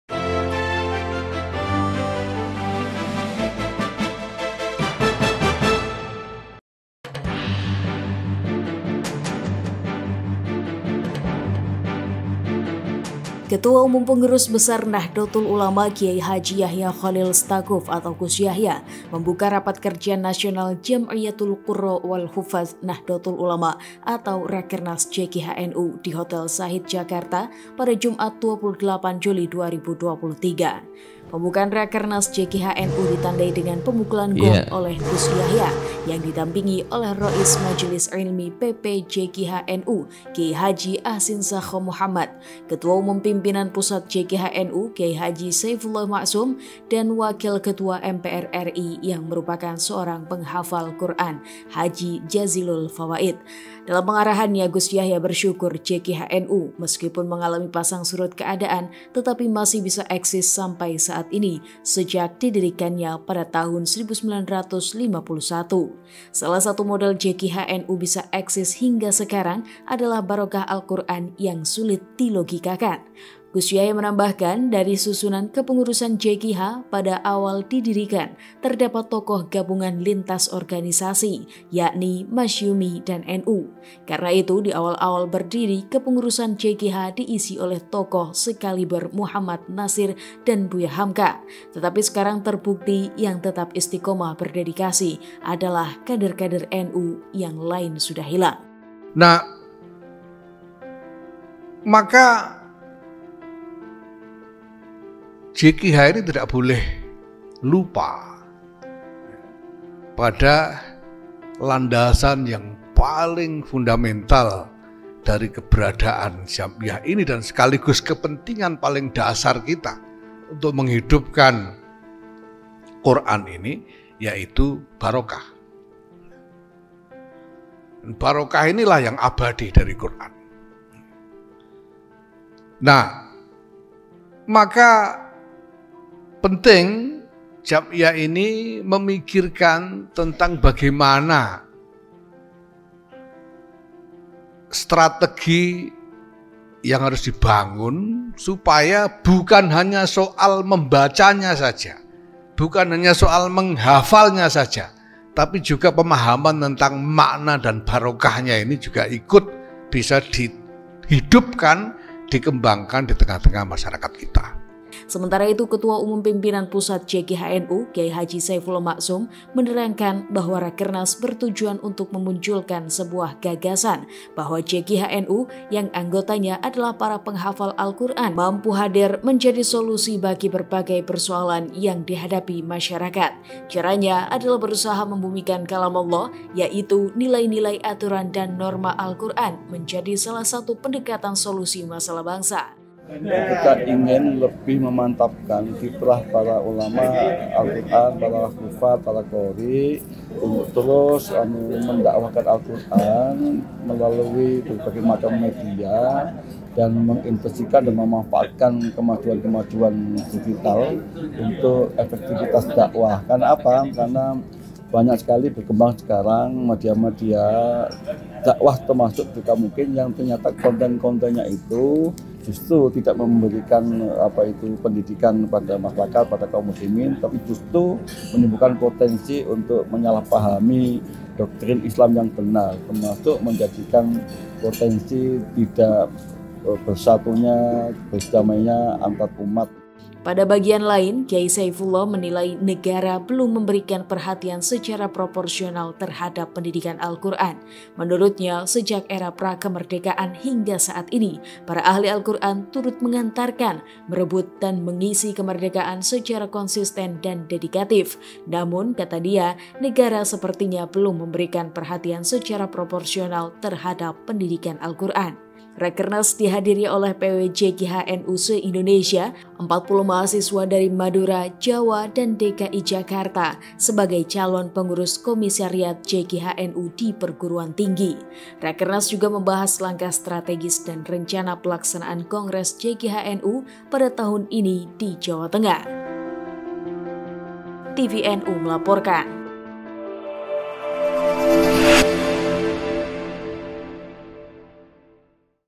Gus Yahya Buka Rakernas JQH NU
Ketua Umum Pengurus Besar Nahdlatul Ulama (PBNU) KH Yahya Cholil Staquf atau Gus Yahya  membuka Rapat Kerja Nasional Jam’iyatul Qurra’ wal Huffazh Nahdlatul Ulama atau Rakernas JQHNU di Hotel Sahid Jakarta, pada Jumat 28 Juli 2023. Pembukaan Rakernas JQHNU ditandai dengan pemukulan gong oleh Gus Yahya yang didampingi Rais Majelis Ilmi PP JQHNU KH Ahsin Sakho Muhammad, Ketua Umum Pimpinan Pusat JQHNU KH Saifullah Ma'shum, dan Wakil Ketua MPR RI yang seorang penghafal Qur'an H Jazilul Fawaid.
Dalam pengarahannya  Gus Yahya bersyukur, JQHNU meskipun mengalami pasang-surut keadaan, tetapi masih bisa eksis sampai saat ini sejak didirikannya pada 1951. Salah satu modal JQHNU bisa eksis sampai sekarang adalah barokah Al-Qur'an yang sulit dilogikakan.  Gus Yahya menambahkan, dari susunan kepengurusan JQH pada awal didirikan, terdapat tokoh gabungan lintas-organisasi yakni Masyumi dan NU. Karena itu, di awal-awal berdiri, kepengurusan JQH diisi oleh tokoh sekaliber Mohammad Natsir dan Buya Hamka. Tetapi sekarang terbukti, yang tetap istiqamah berdedikasi adalah kader-kader NU, yang lain sudah hilang.

CREDIT:
Kameramen:  Saripudin
Reporter: Muhamad Saefullah
Penulis naskah: Muhamad Saefullah
VO: Oppie
Editor: Sarif